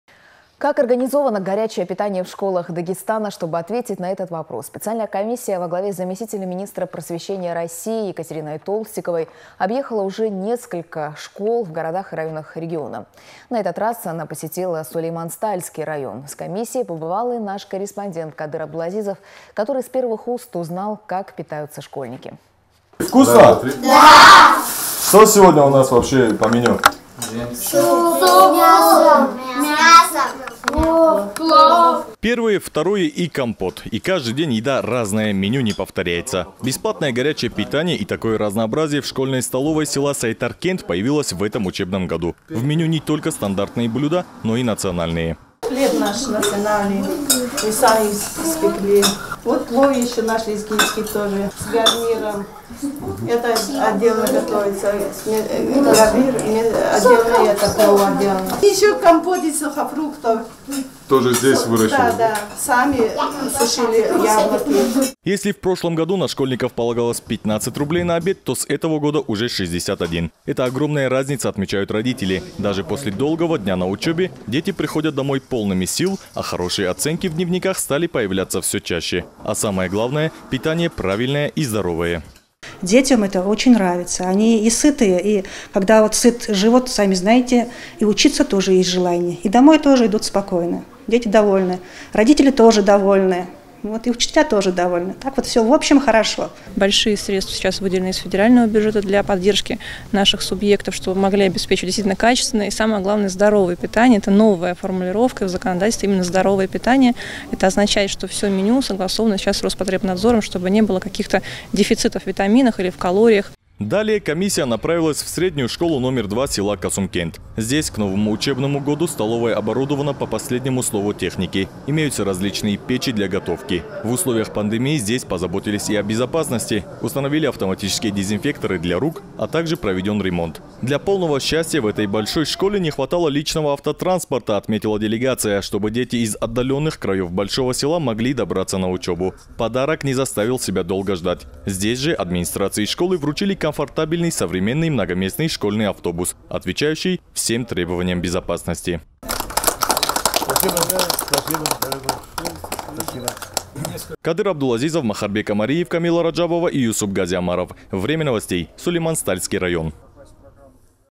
Как организовано горячее питание в школах Дагестана?
Первое, второе и компот. И каждый день еда разная – меню не повторяется. Бесплатное горячее питания и такое разнообразие в школьной столовой села Сайтаркент появилось в этом учебном году. В меню не только стандартные блюда, но и национальные.